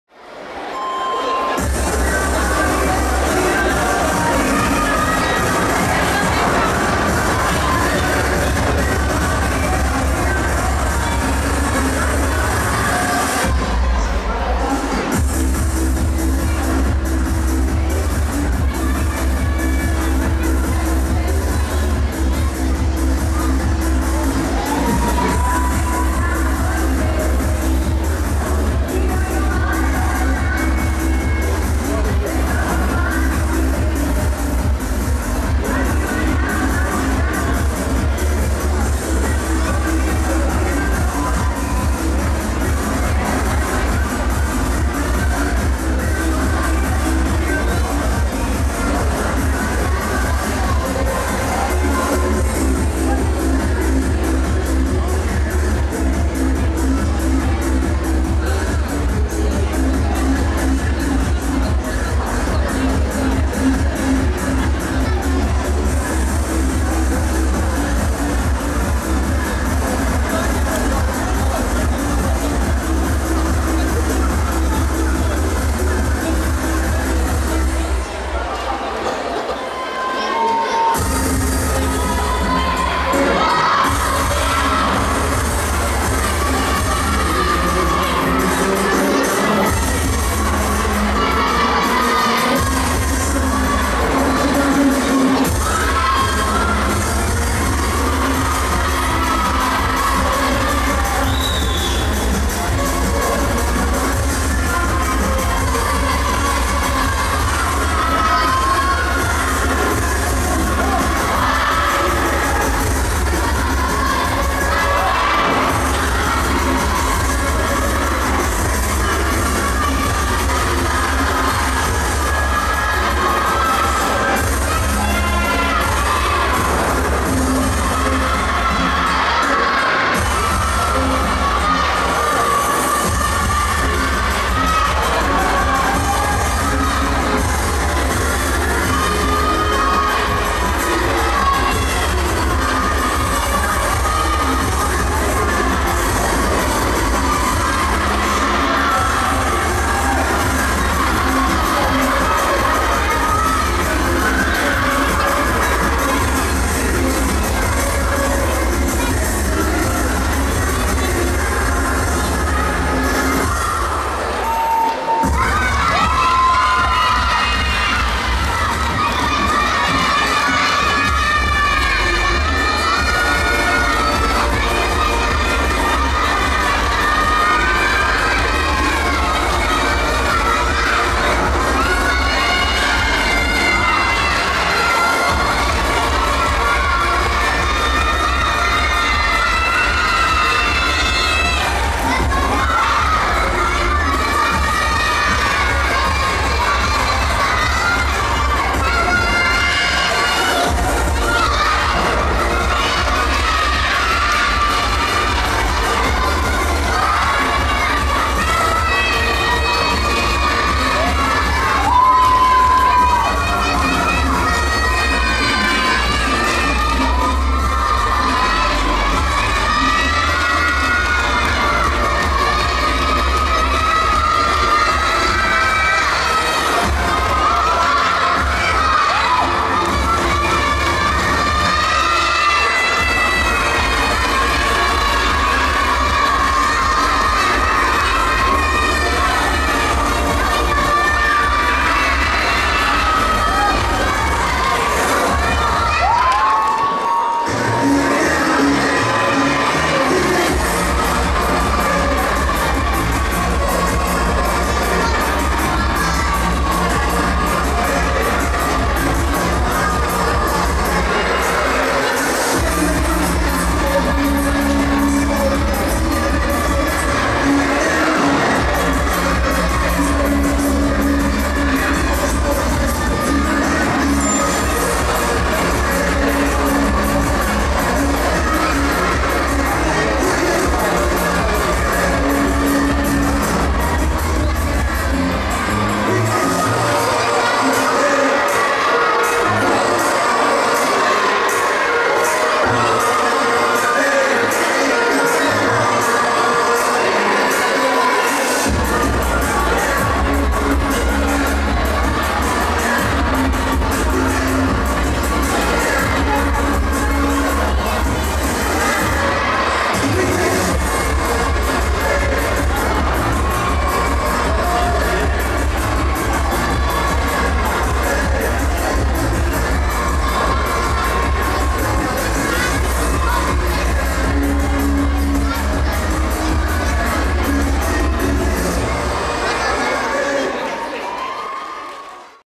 Group 18+ KALININGRAD OPEN CUP 2019
Всероссийские соревнования по спортивной аэробике «Балтийский берег» и открытый турнир «Kaliningrad Open Cup 2019»
Организатор: РОО "Федерация спортивной аэробики Калининградской области"
Директор соревнований Анна Дворецкая
г. Калининград Многоцелевой спортивный зал "СОЗВЕЗДИЕ" 30 апреля 2019 года